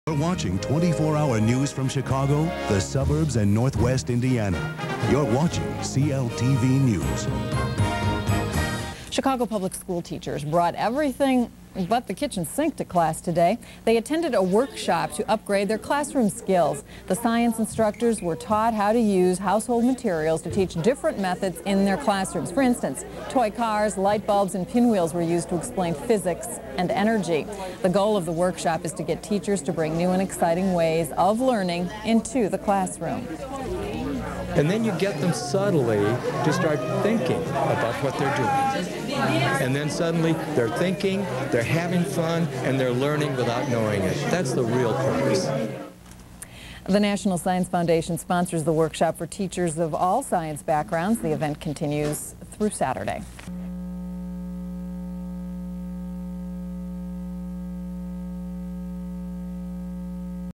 CLTV Report On Summer Workshop Pioneered by Professor Zafra Lerman
CLTV Covers Summer Workshops at Columbia College's Science Institute. (July 1998)